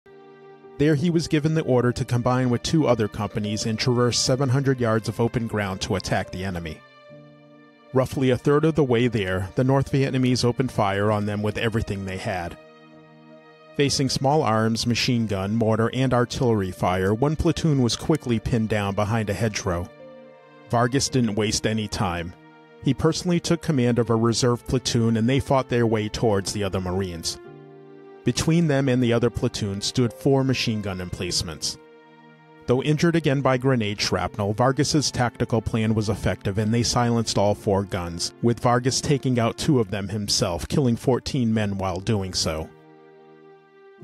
US Marine Corps Col Jay Vargas: Medal of Honor in the Vietnam War
Colonel Jay R. Vargas dedicated his life to the United States Marine Corps.  His defining moment came during the Vietnam War's Battle of Dai Do in 1968, where he served as a company commander.  Vargas displayed exceptional leadership and courage, earning him the Medal of Honor, the highest military decoration in the U.S.  After Vietnam, he continued his military service, holding various command and staff positions until his retirement in 1992.  Colonel Vargas's illustrious career is marked by a legacy of valor, selflessness, and an enduring commitment to the principles of duty and valor.

Photos courtesy of Wikimedia Commons

🔹 Learn more about Jay Vargas and other American heroes on our